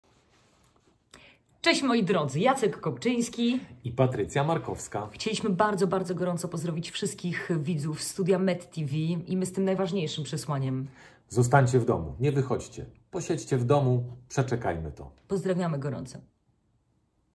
Patrycja Markowska i Jacek Kopczyński mówią Zostań w domu...
zostanwdomu - wiadmość od Patrycji Markowskiej i Jacka Kopczyńskiego